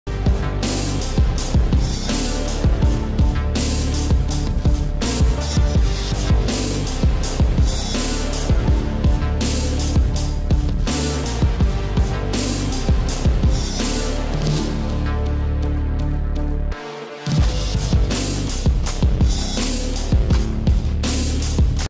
Výzva I - sundat si mikinu v pozici na čtyřech
1. výzva pro naše hráče: sundat si mikinu v pozici na čtyřech - viz video
* video je převzato z Českého hokeje